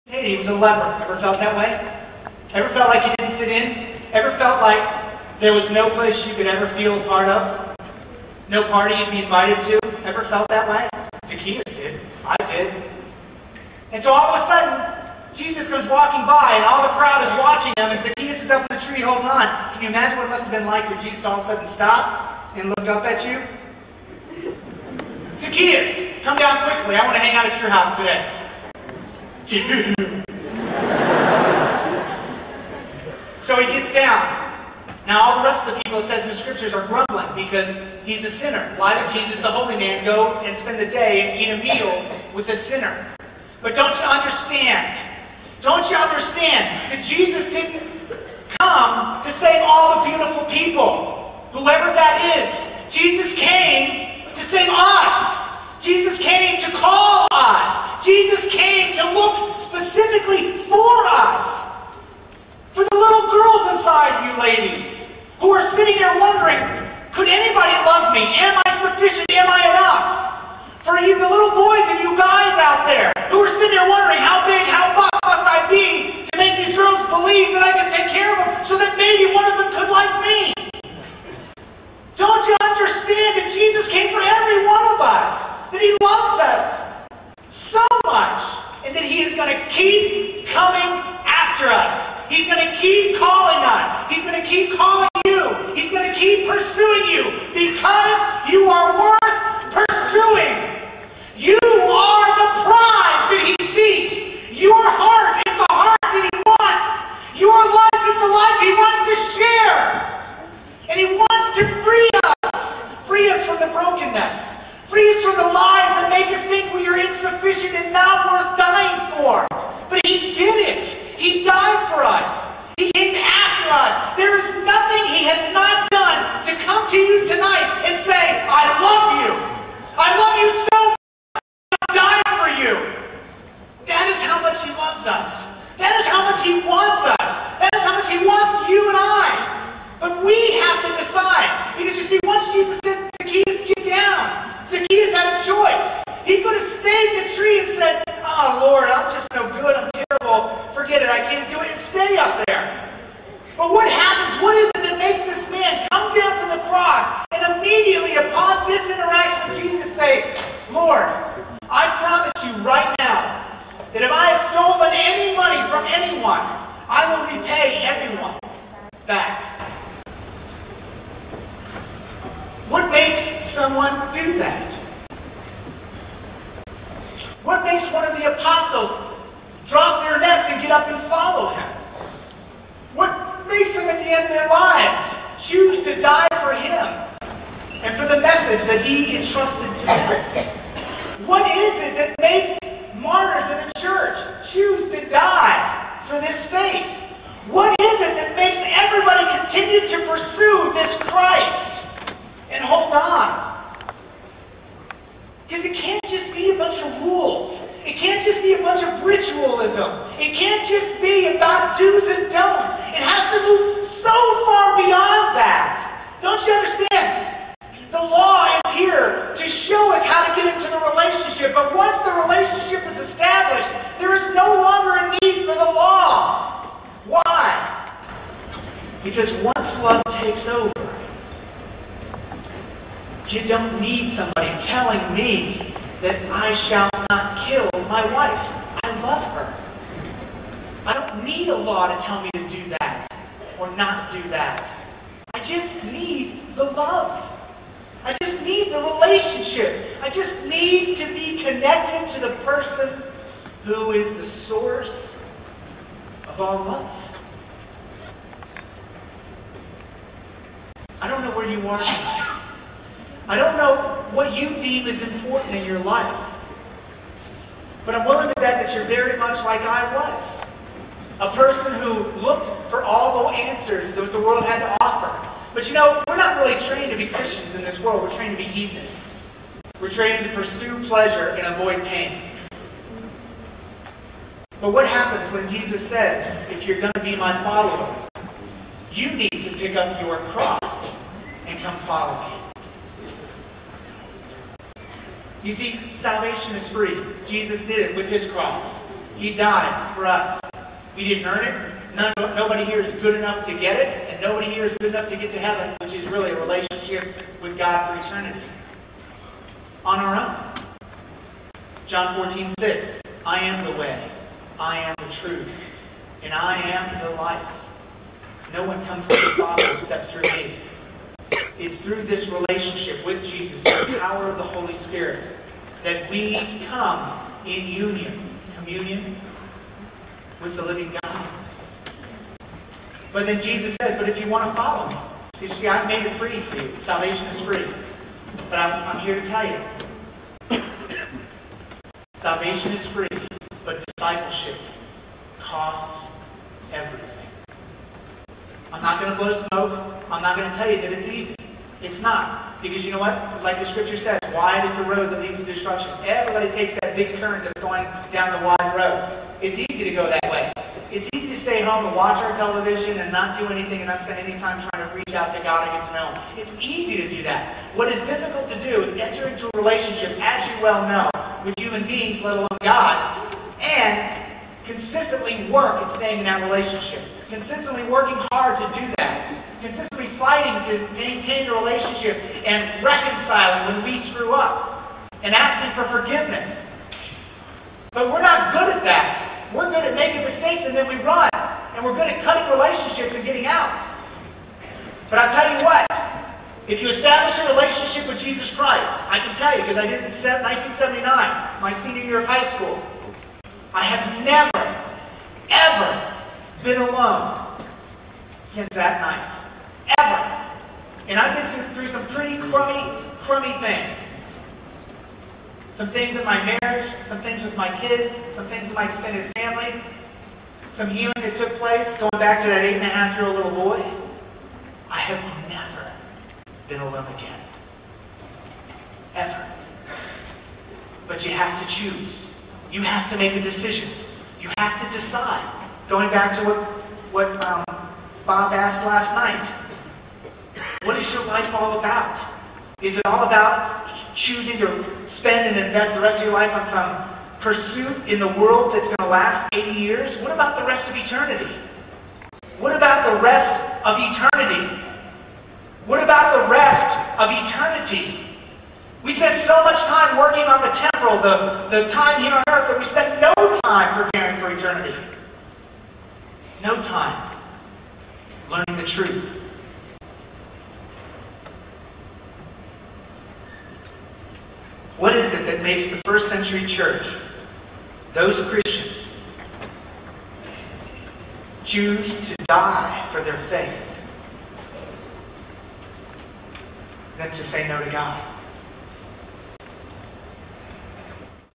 Deacon Ralph Poyo at Steubenville West 2009
Deacon Ralph Poyo's talk at Steubenville West 2009

Saint Anthony of Padua Life Teen in Gardena, CA
at Steubenville West 2009